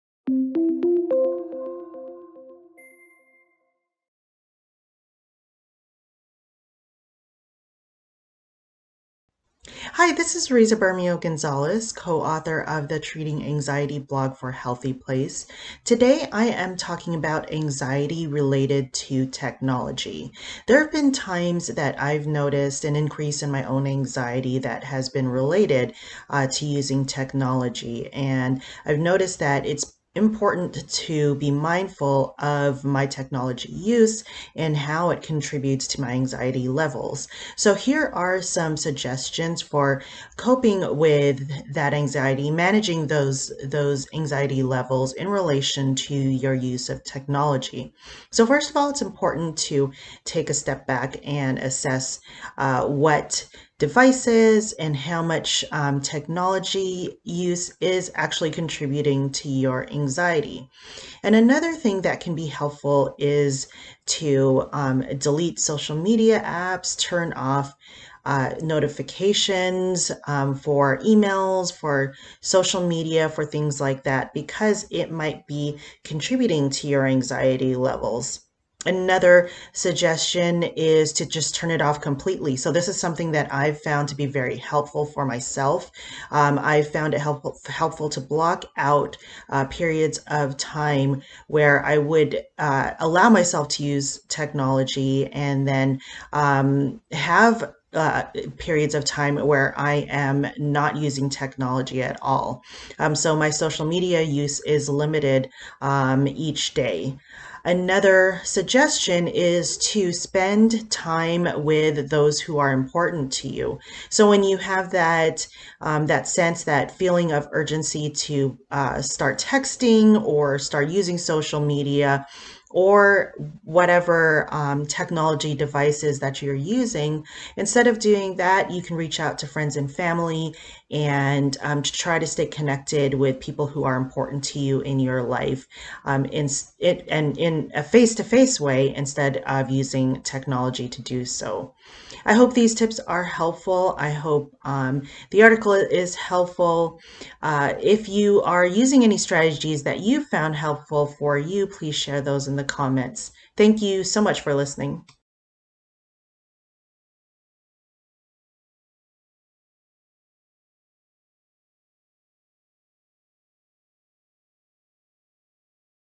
Ways to Cope with Technology-Related Anxiety | HealthyPlace
Technology can sometimes be related to anxiety. Learn how Rizza copes with her technology-related anxiety and why it can help you, too. Watch this.
'When Technology Makes You Anxious'

Anxiety can be related to technology use. Find out what anxiety related to technology can look like and strategies you can use to cope at HealthyPlace.